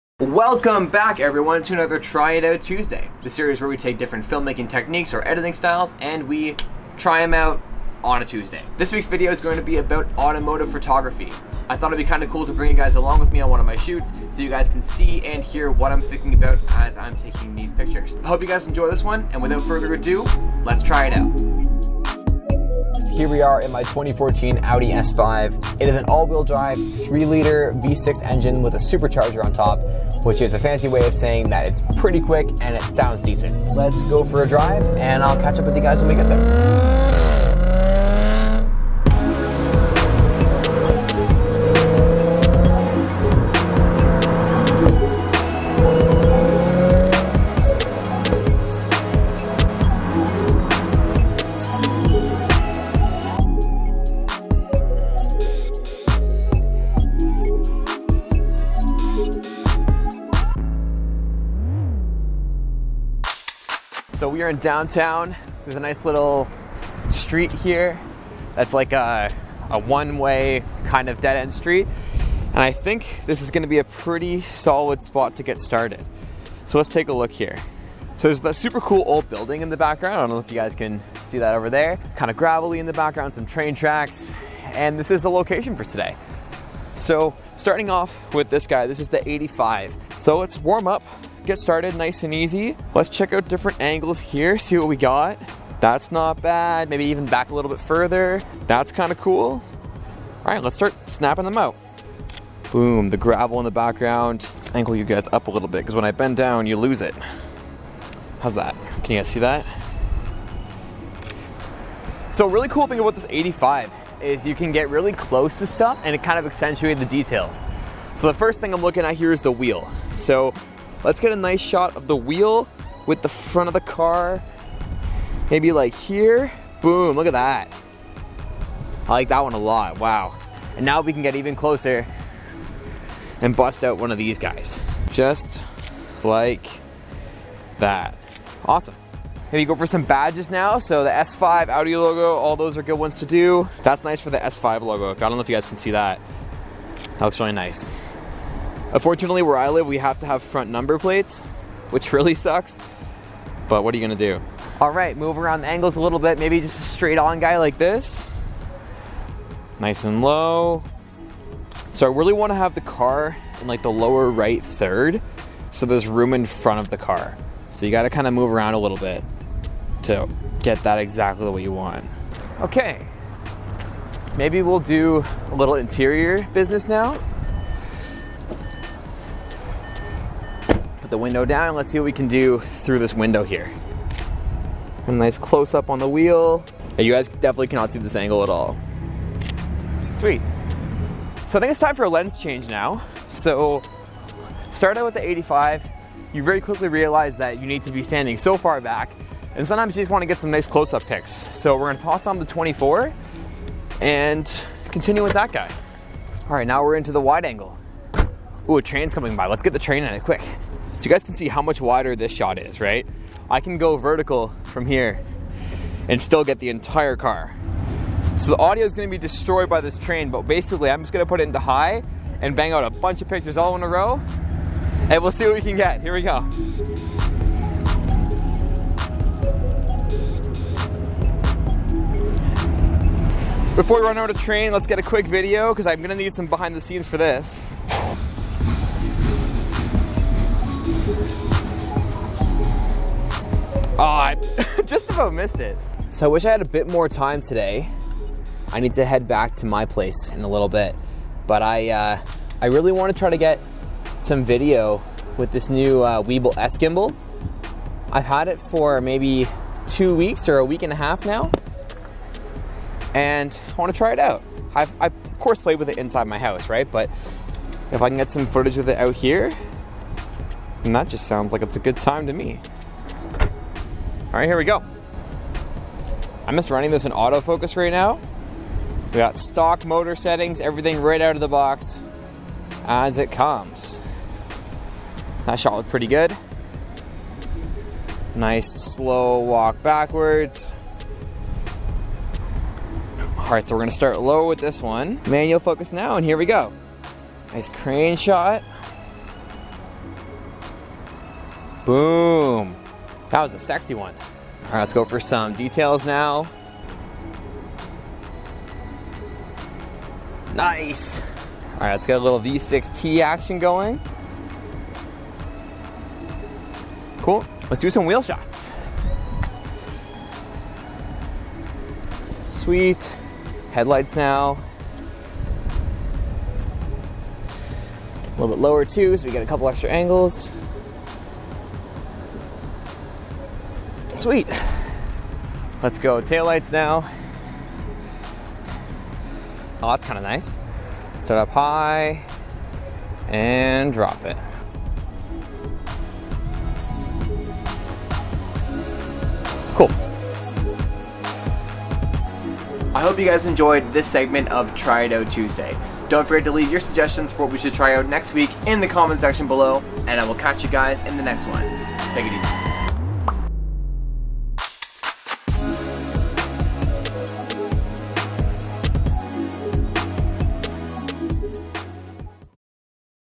6 Minutes of Pure Car Photography
6 Minutes of Pure Car Photography

This is the second installment in a new series I’m calling Try It Out Tuesday! The series where we try out different film making techniques, tips and effects together. Some will be good, some will be bad. This week I bring you along with me to shoot some car photos and videos featuring my Audi S5. We take pictures with the sony 85 1.8, the sigma 24 1.4 as well as use the new Zhiyun weebill S gimbal. All content was captured using the Sony a7iii and insta 360 one r.

The gear I use to make most videos:

Main Camera
Wide Angle Lens
Standard Zoom Lens
Camera Monitor
Mavic Drone